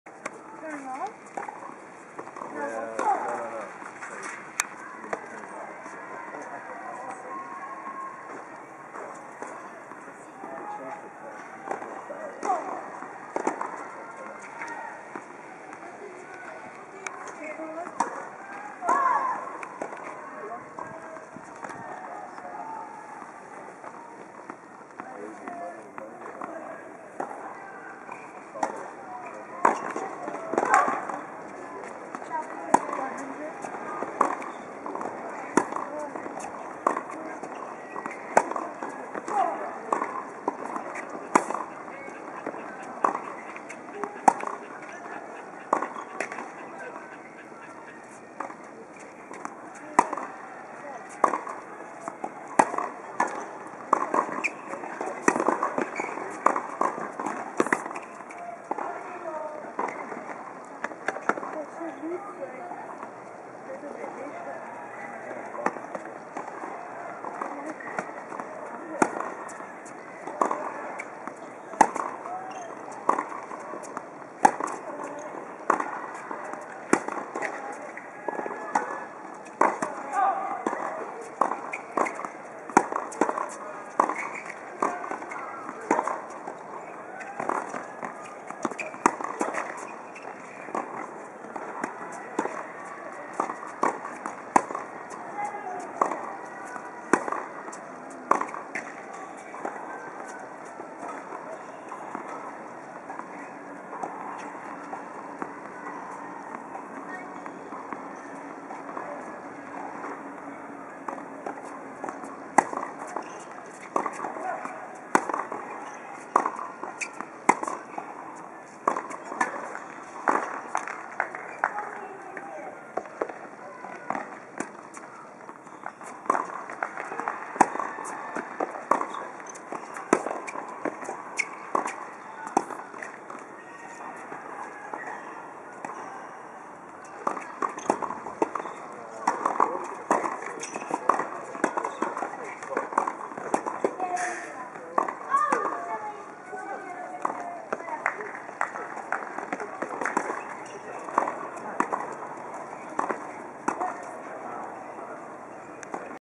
ルクシカ・クムクム対ザン・リン試合開始 カンガルーカップ2012
ルクシカ・クムクム（タイランド）ＶＳザン・リン（香港）戦、試合開始。